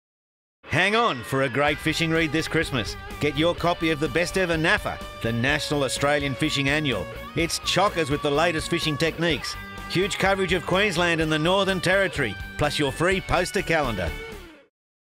National Australian Fishing Annual Advert
Short advert for NAFA 18 magazine now available from newsagents and BCF stores around Australia - December 2009